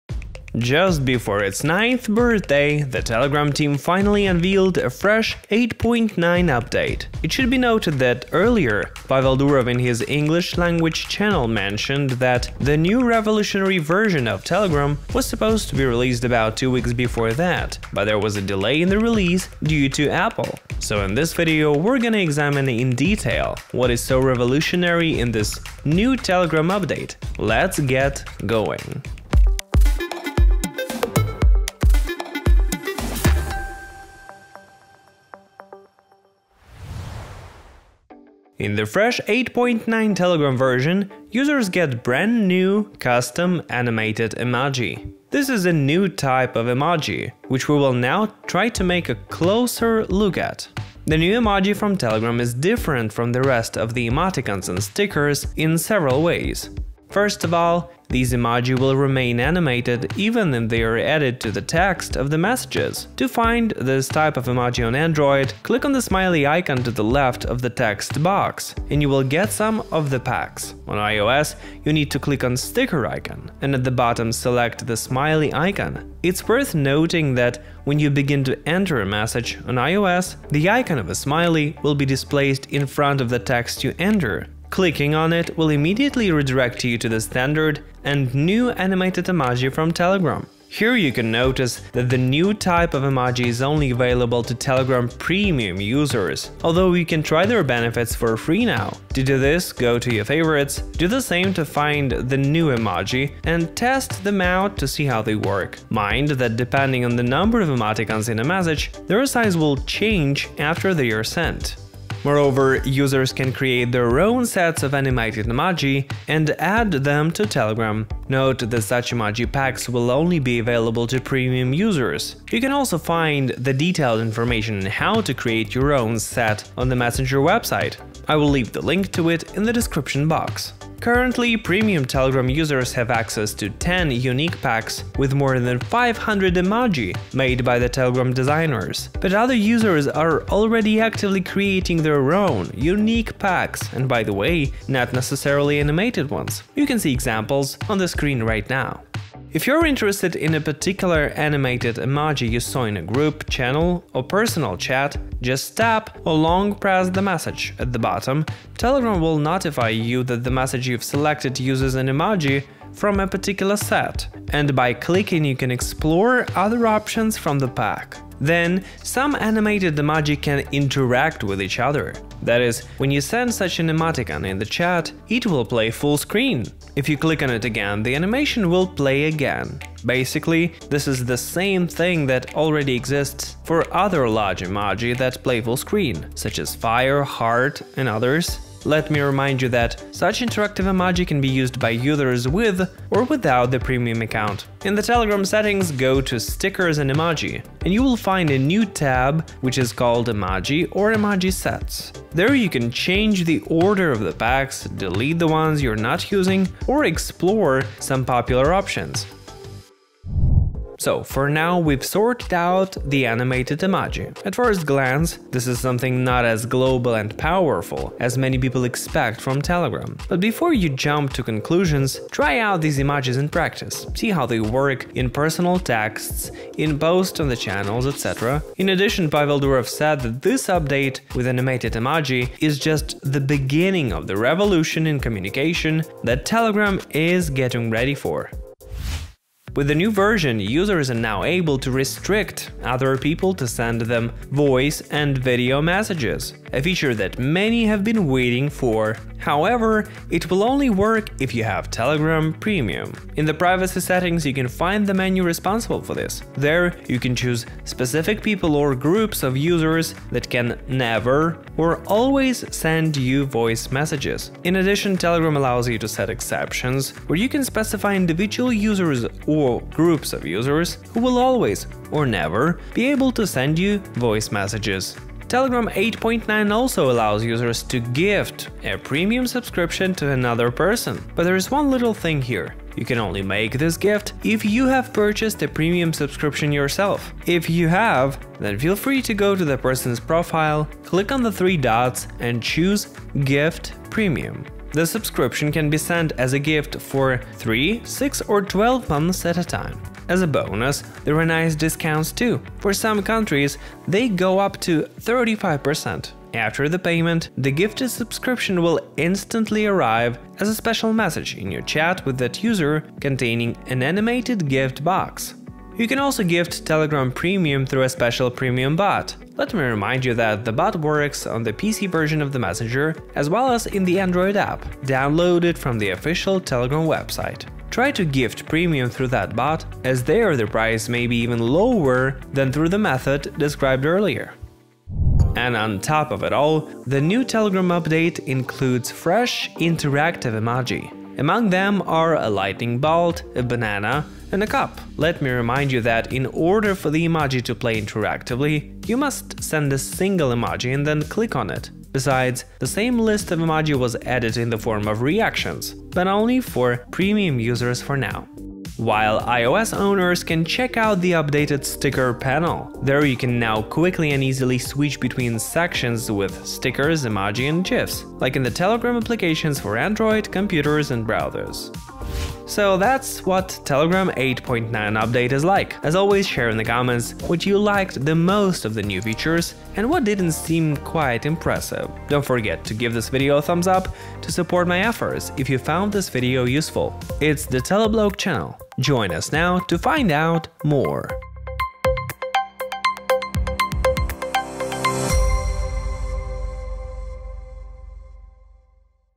NEW Telegram 8.9: RESTRICT VOICE MESSAGES, PREMIUM TELEGRAM AS A GIFT, CUSTOM EMOJI
Just before its ninth birthday, the Telegram team finally unveiled a fresh 8.9 update. It should be noted that earlier Pavel Durov in his English-language channel mentioned that the new, revolutionary version of Telegram app was supposed to be released about two weeks before that, but there was a delay in the release due to Apple. In this video, we will examine in detail what is so revolutionary in the new Telegram update. Let's get going.

Don't miss:
Telegram Premium: HOW to GET it CHEAPER that others